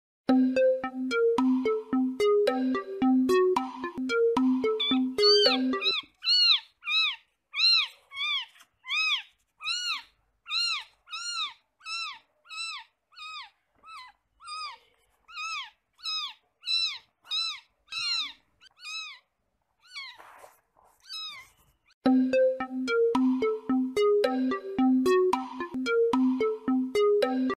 Baby Pika Getting Used To Her Very First Box :')
she was trying to make herself comfortable in her very first box :)